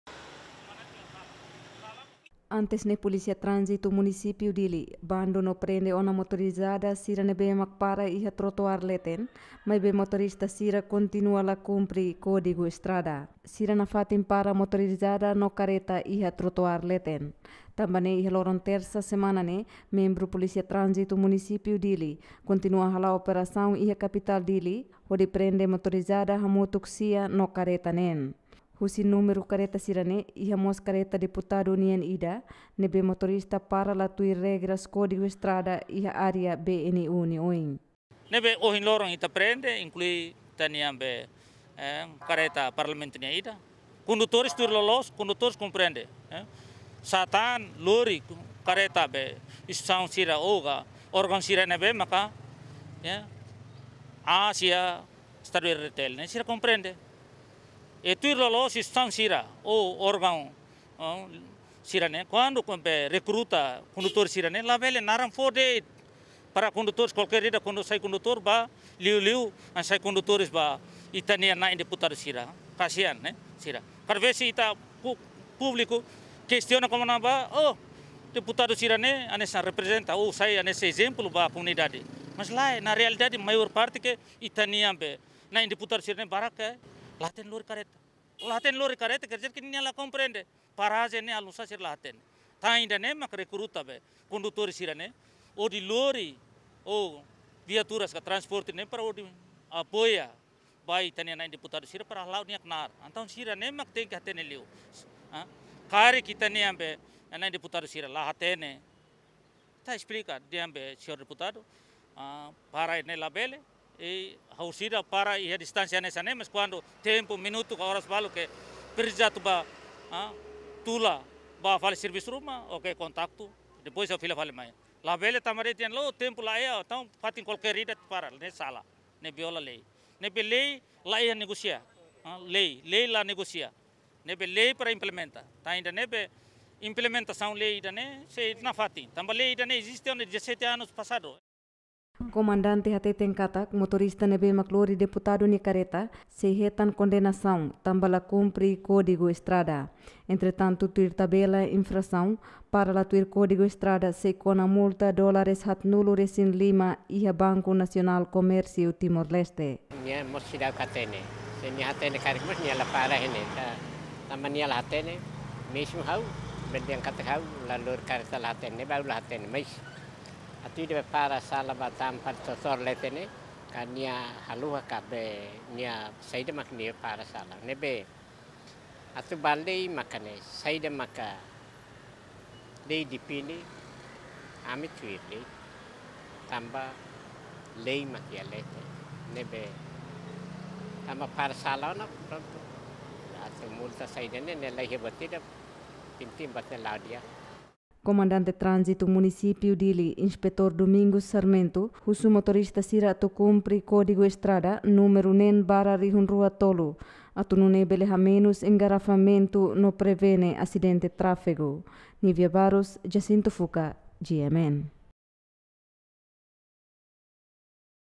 Polisia Tranzitu Munispiu Díli Kontinua Prende Motorizadas no Kareta
Polisia Tranzitu Munisipiu Díli Kontinua Prende Motorizadas no Kareta